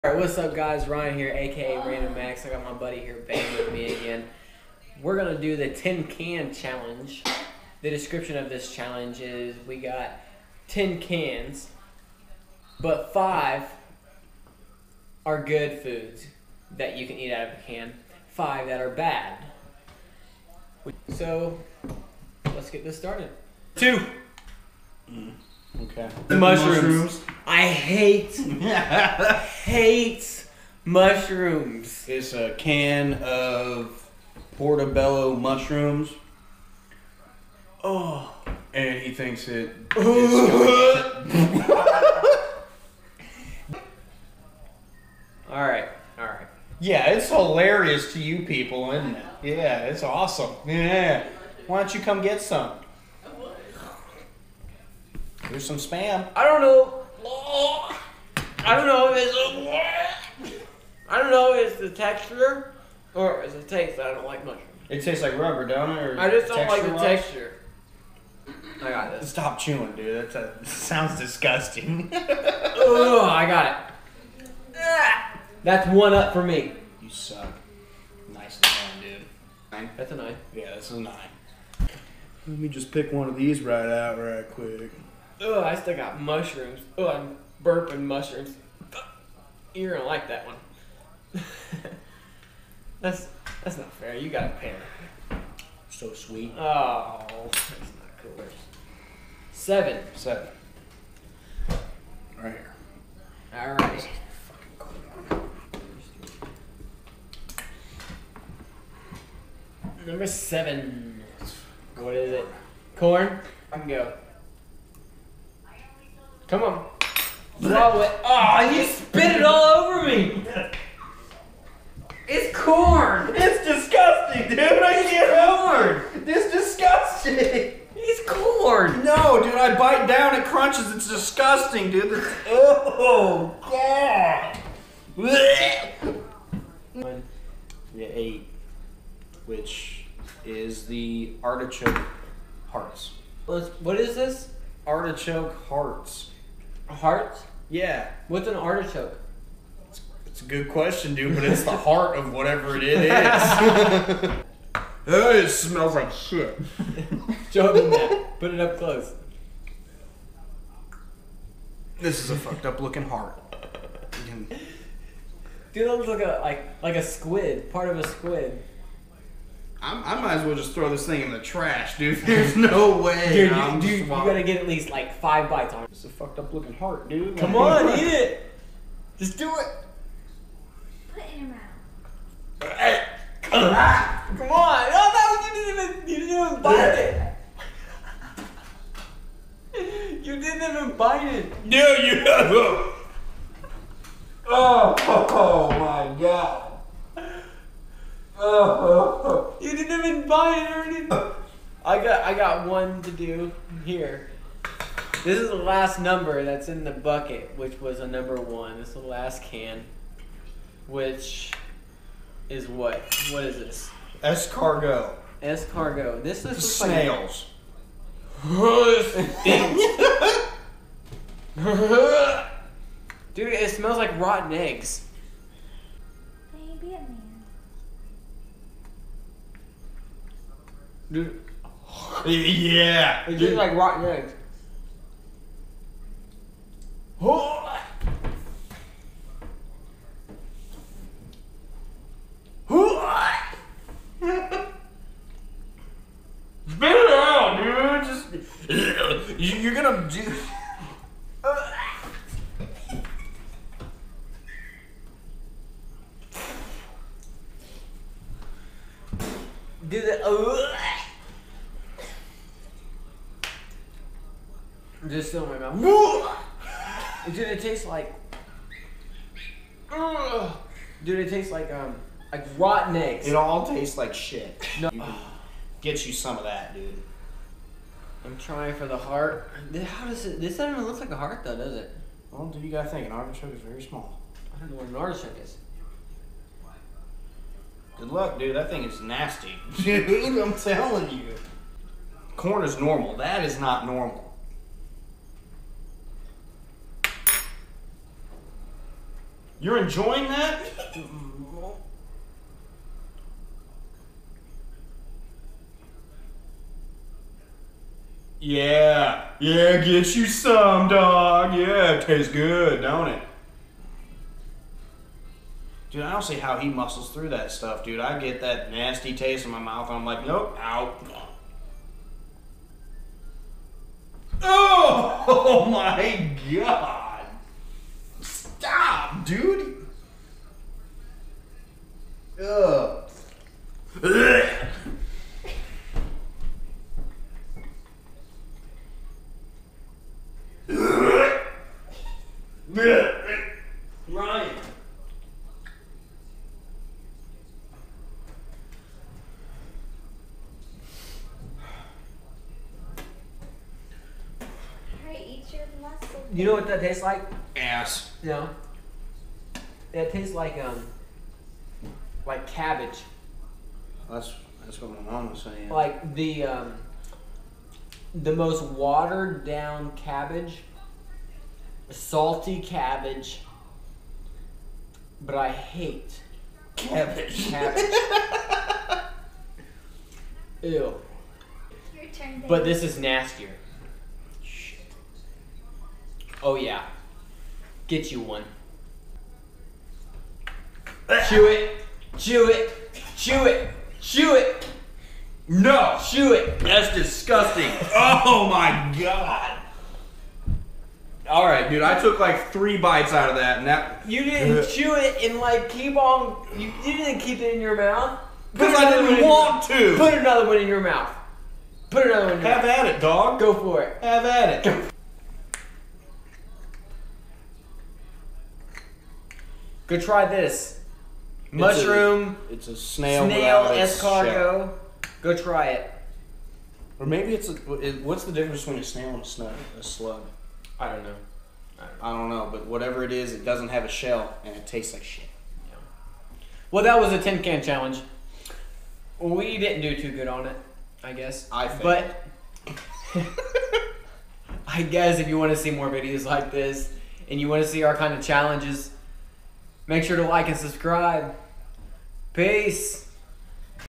Eating HEARTS and SNAILS(Tin Can Challenge)
Description:
The snails were the most disgusting thing iv ever put in my mouth
give me a thumbs up for trying
leave comments what u thought of the challenge